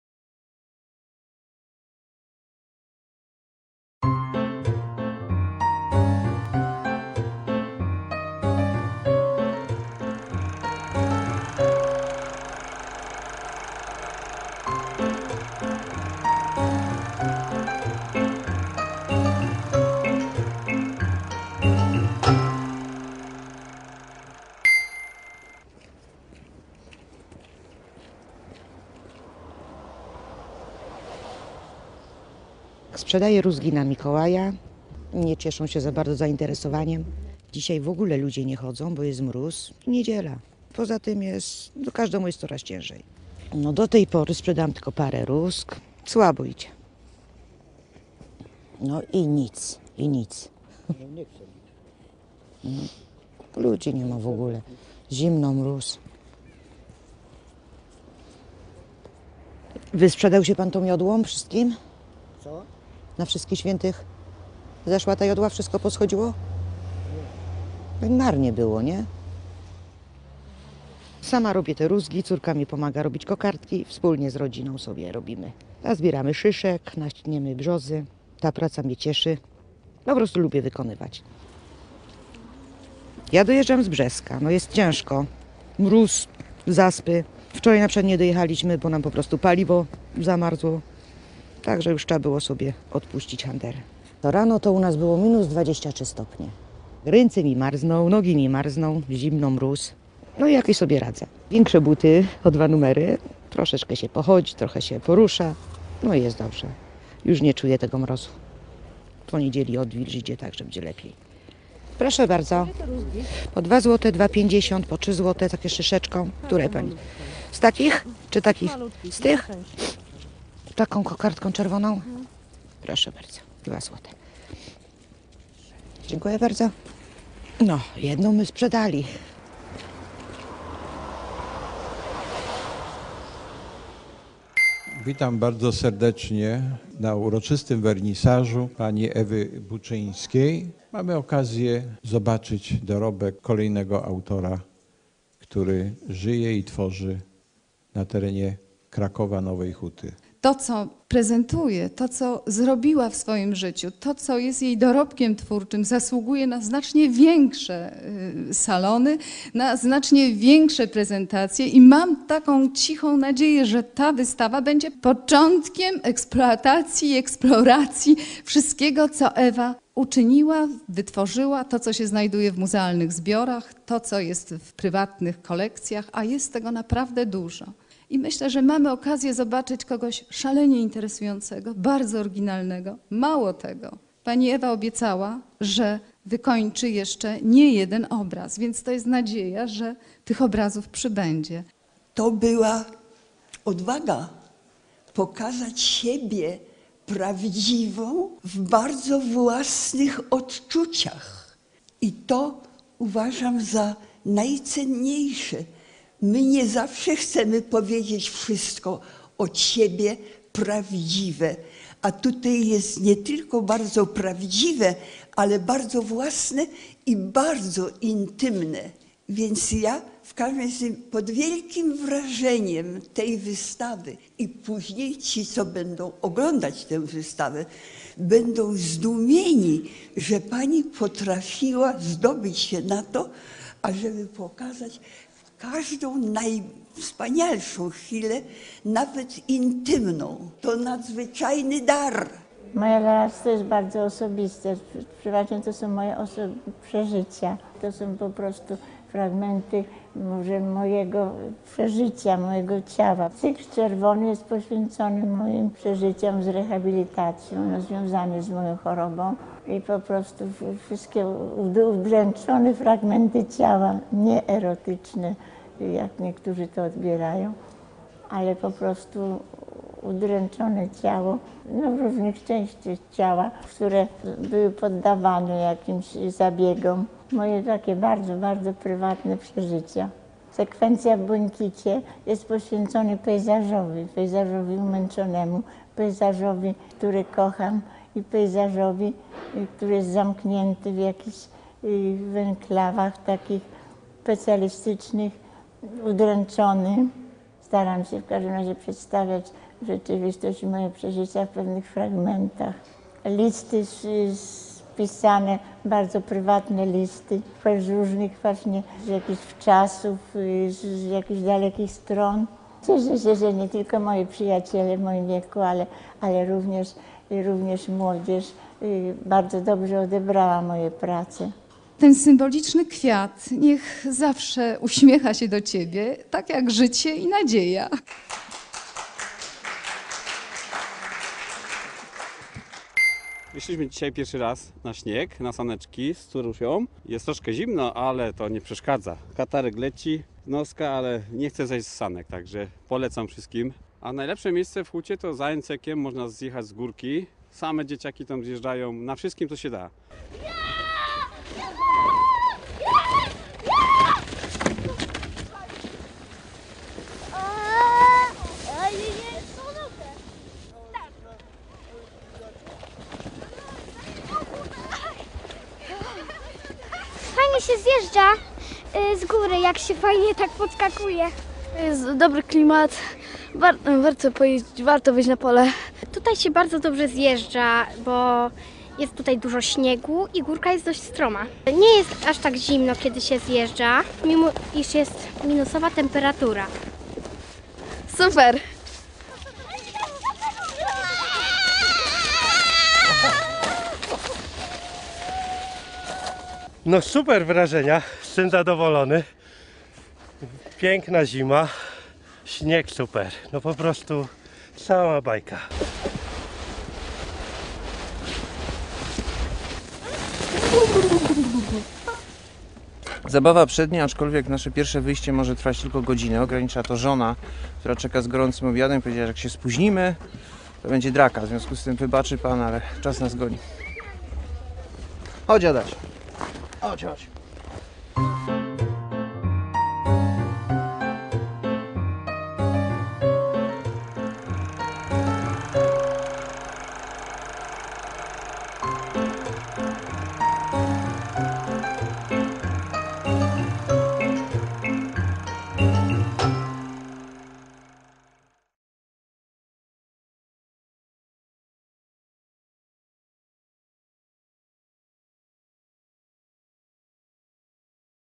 Nowohucka Kronika Filmowa nr 232
SPOTKANIA W NOWEJ HUCIE: MAŁGORZATA SZCZUPAK – uliczny handlowiec
W Nowej Hucie przy Placu Bieńczyckim pani Ewa sprzedaje rózgi na św. Mikołaja. O dziwo – nie cieszą się zainteresowaniem. Bo mróz, zimno, a w ogóle to ludziom jest coraz ciężej. Ale Pani Ewa lubi swoją pracę Przyjeżdża tu aż z Brzeska. A rózgi robi wraz z córką – takie pozłacane, z szyszką i kokardką.

ŚWIAT EWY BUCZYŃSKIEJ
W Nowohuckim Centrum Kultury otwarto wystawę prac Ewy Buczyńskiej "Znaki czasu". W trakcie wernisażu o autorce opowiadały Joanna Lewińska (kurator wystawy) oraz Halina Bohdanowicz (wieloletnia opiekunka nowohuckich artystów). Podkreślały niezwykłą intymność twórczości Ewy Buczyńskiej, odwagę mówienia prawdy o sobie. Sama autorka również nie boi się przyznać, że jej prace są bardzo osobiste – cykl czerwony związany jest z chorobą, a pokazane w różnych ujęciach ciało to nie emanacja erotyzmu, ale cierpienia. Cykl w błękitach to udręczony pejzaż.

PIERWSZY ŚNIEG
Pierwszy śnieg to okazja do pierwszych wypraw na sanki. Najlepiej wybrać się w okolice NCK, bo tam można pozjeżdżać z górki – na wszystkim, co się da. Spotkane tam Natalia, Iza i Tosia udowadniają, że jak się szybko zjeżdża, to wcale nie jest zimno. A i tradycyjnego orzełka na śniegu można wyklepać. Towarzyszący pociechom tatusiowie też wyglądają na zadowolonych?